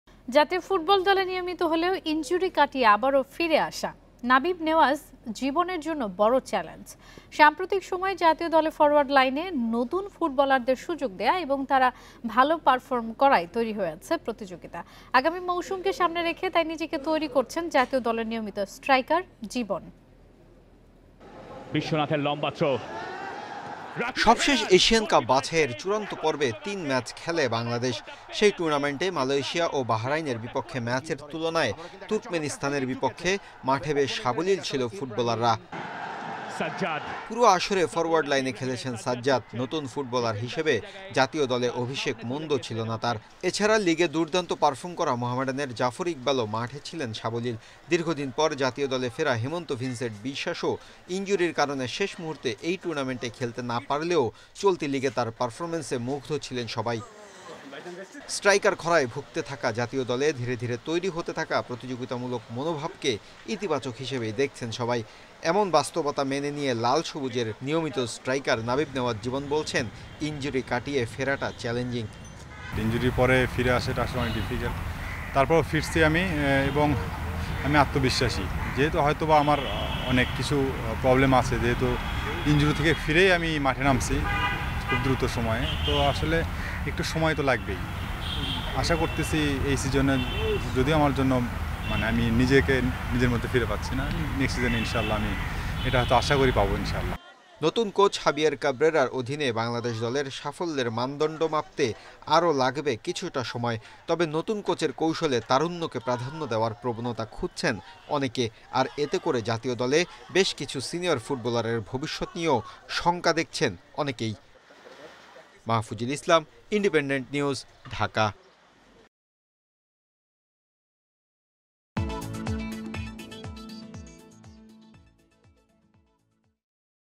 ইনজুরি কাটিয়ে দ্রুত ফেরা চ্যালেঞ্জিং | Footballer Jibon | BD Football
জাতীয় ফুটবল দলে নিয়মিত হলেও ইনজুরি কাটিয়ে আবারো ফিরে আসা, নাবিব নেওয়াজ জীবনের জন্য বড় চ্যালেঞ্জ। সাম্প্রতিক সময়ে জাতীয় দলে ফরোয়ার্ড লাইনে নতুন ফুটবলারদের সুযোগ দেয়া এবং তারা ভালো পারফর্ম করায় তৈরি হয়েছে প্রতিযোগিতা। আগামী মৌসুমকে সামনে রেখে তাই নিজেকে তৈরি করছেন জাতীয় দলের নিয়মিত স্ট্রাইকার জীবন।